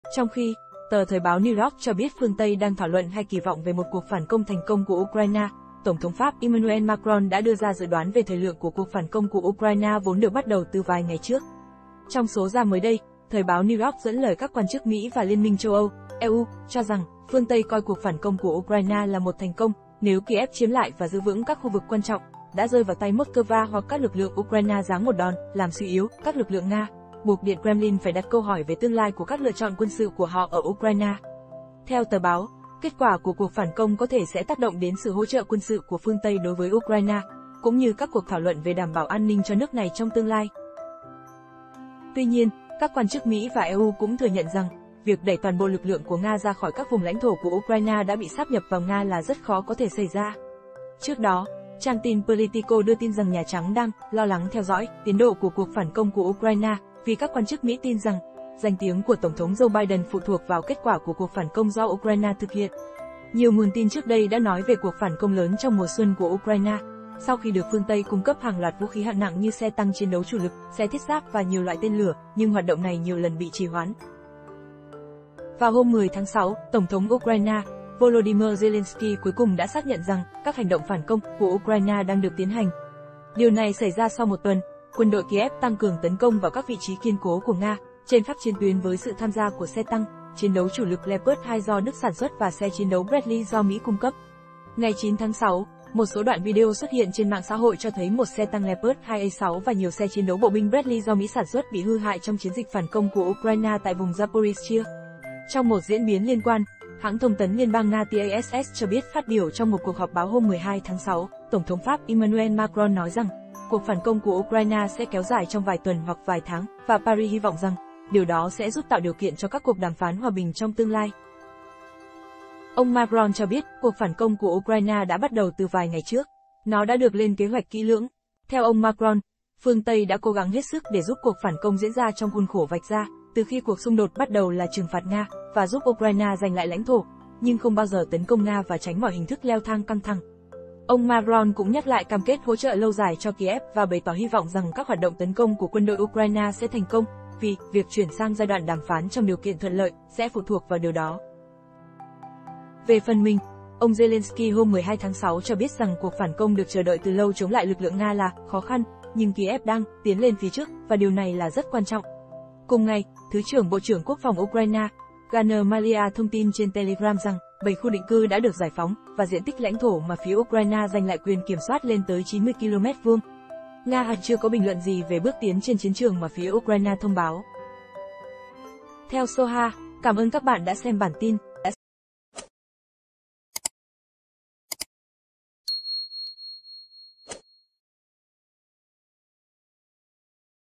KỲ VỌNG CỦA PHƯƠNG TÂY VÀO VIỆC UKRAINE PHẢN CÔNG LẦN NÀY - TỔNG THỐNG PHÁP DỰ ĐOÁN THỜI GIAN CÓ THỂ
Trong khi, tờ Thời báo New York cho biết phương Tây đang thảo luận hai kỳ vọng về một cuộc phản công thành công của Ukraine, Tổng thống Pháp Emmanuel Macron đã đưa ra dự đoán về thời lượng của cuộc phản công của Ukraine vốn được bắt đầu từ vài ngày trước.